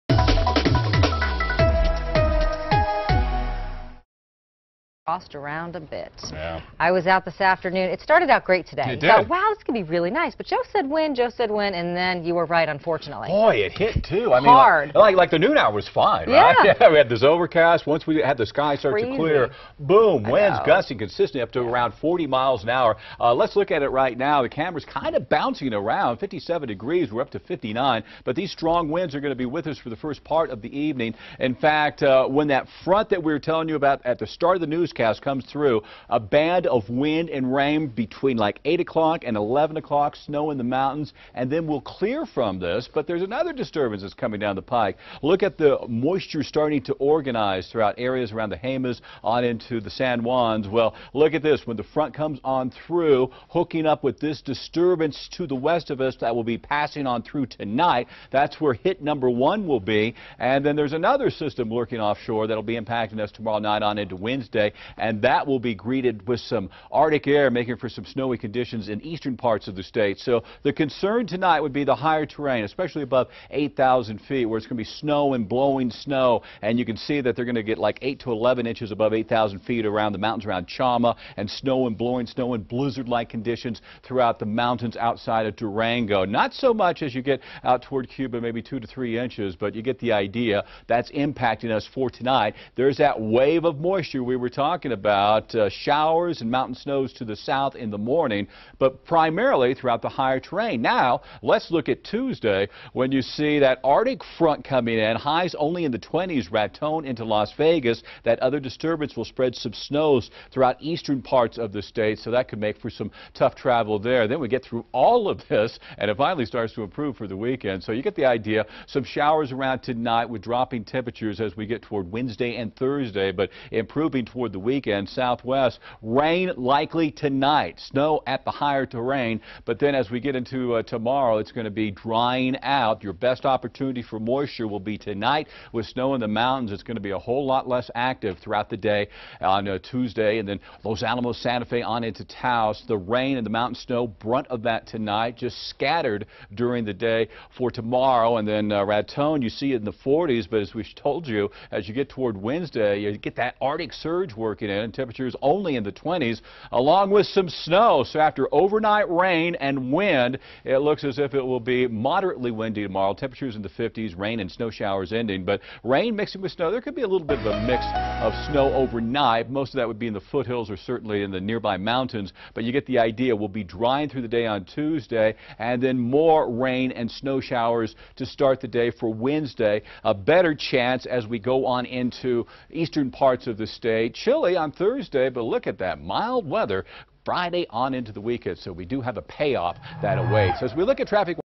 NM looks ahead to pleasant weekend
There is a chance this upcoming weekend will be a mild and pleasant one in New Mexico.